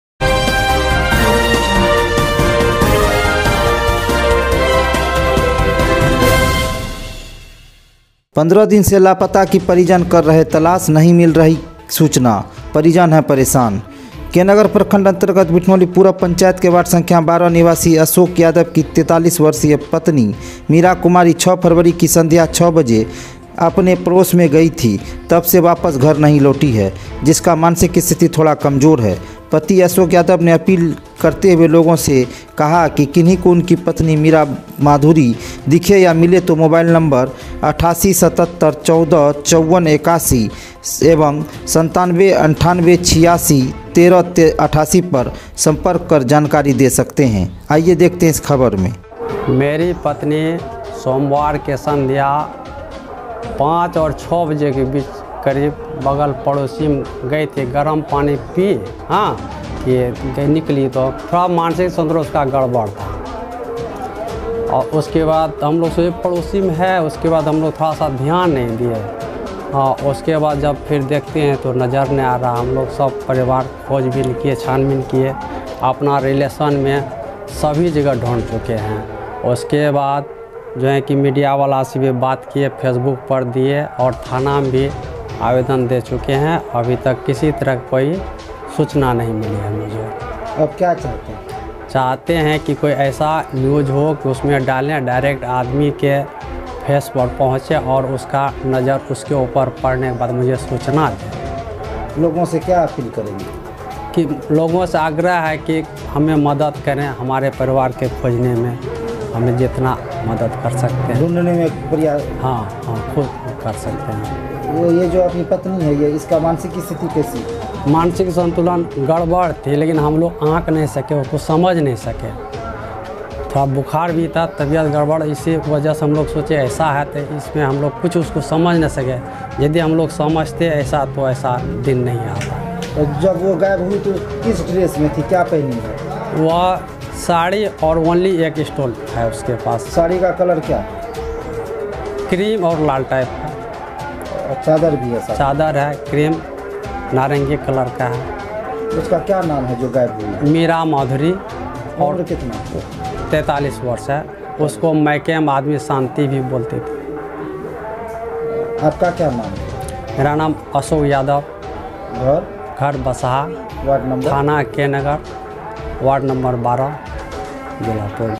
15 दिन से लापता पत्नी की तलाश में पति के साथ परिजन है परेशान नही मिल रही सूचना तलाश में करे सहयोग
देश का नम्बर-1 न्यूज चैनल पर आप तमाम देश वासियों का स्वागत है। निष्पक्ष व निर्भीक पत्रकारिता करने के लिए खबर_हर_पल YouTube चैनल व टीम के सदस्यों को सपोर्ट करें।

👉🏻टीम Khaber Har Pal नेटवर्क.